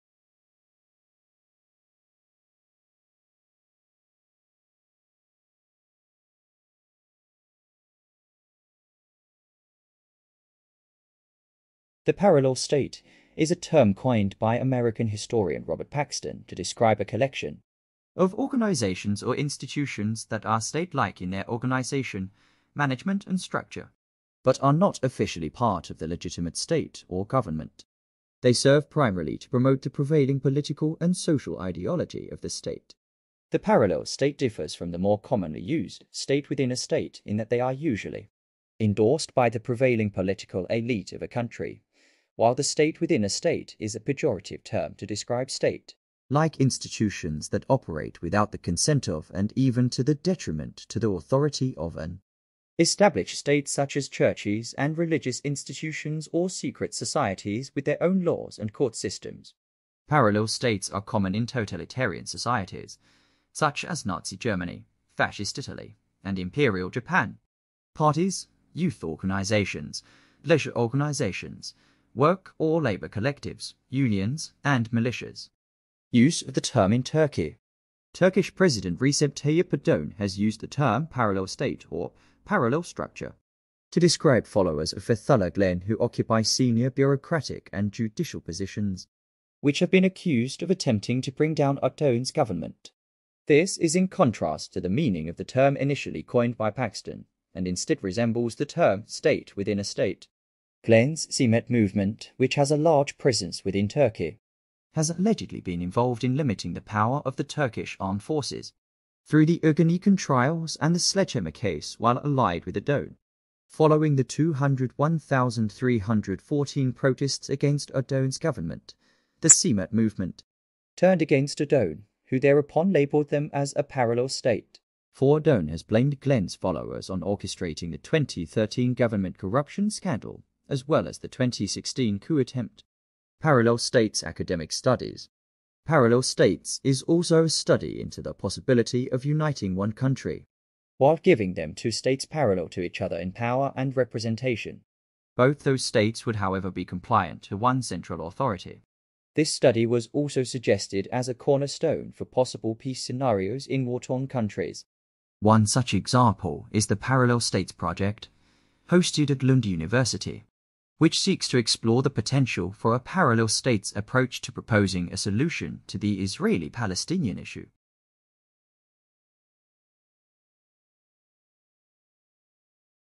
What Are Parallel State
Explore the enigmatic realm of the Parallel State in our latest video. 🌐🔍 Uncover the intricacies of dual power structures and navigate through the complexities that shape this parallel reality. Join us on a journey of understanding as we decode the dynamics that coexist alongside conventional systems. Are we witnessing a shift in power? Find out as we delve deep into the world of the Parallel State.